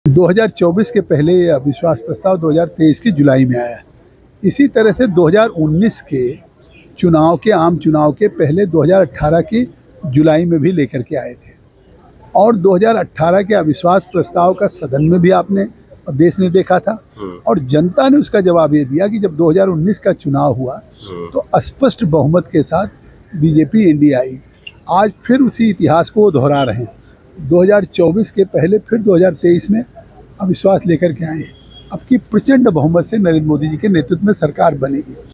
2024 में भी NDA की जीत का इतिहास दोहराया जाएगा: Jagdambika Pal | abpliveshorts | ABP LIVE
बीजेपी सांसद जगदंबिका पाल ने कहा कि 2019 के चुनावों से पहले 2018 में भी विपक्ष अविश्वास प्रस्ताव लेकर आया था और उसके बाद जनता ने 2019 में बीजेपी और मोदी सरकार का भरोसा जताया एक बार फिर इतिहास उसी को दोहरा रहा है और 2024 में भी उसी को दोहराया जाएगा।।